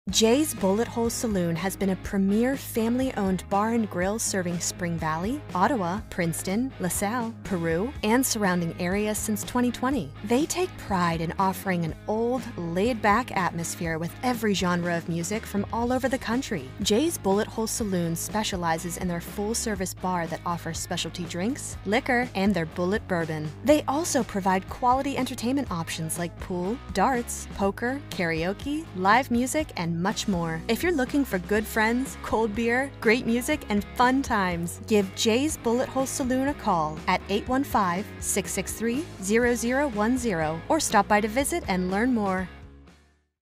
Jay’s Bullet Hole Saloon in Spring Valley, IL
Jays Bullet Hole Saloon has been a premier family-owned bar and grill serving Spring Valley, Ottawa, Princeton, LaSalle, Peru and surrounding areas since 2020. They take pride in offering an old, laid back atmosphere with every genre of music from all over the country. Jays Bullet Hole Saloon specializes in their full service bar that offers specialty drinks, liquor, and their bullet bourbon. They also provide quality entertainment options like pool, darts, poker, karaoke, live music, and much more. If you’re looking for good friends, cold beer, great music and fun times, give Jays Bullet Hole Saloon a call at or stop by to visit and learn more.